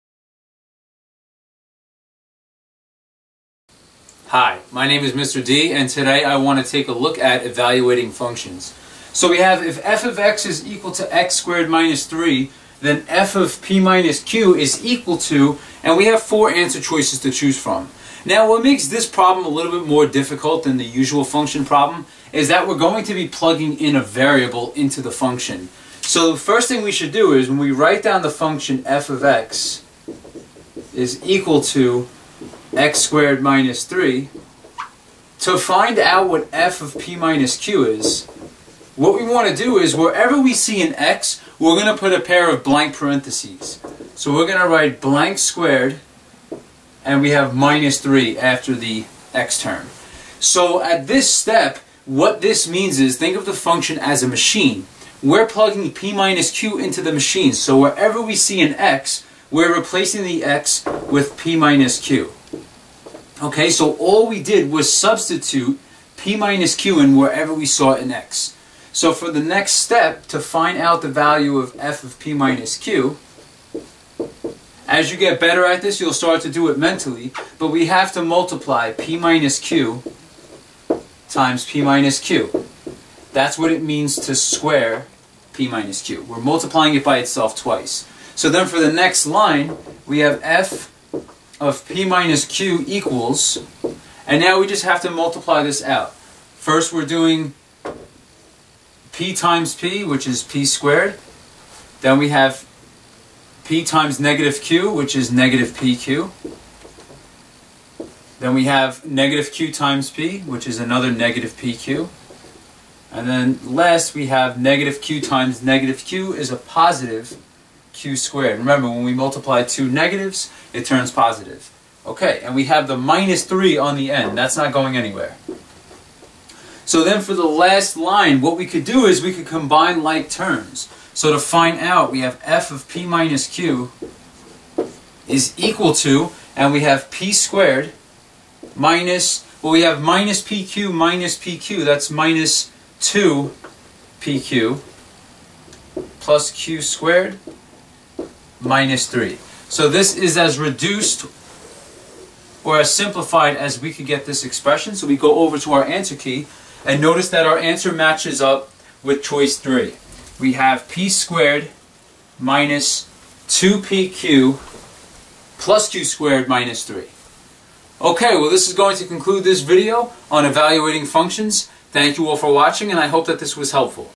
Evaluating Functions (with variables)
This video focuses on evaluating functions with variables. In particular, I show students how to plug a variable expression into a function of x. Next, I carefully show students how to multiply two binomials to simplify the function.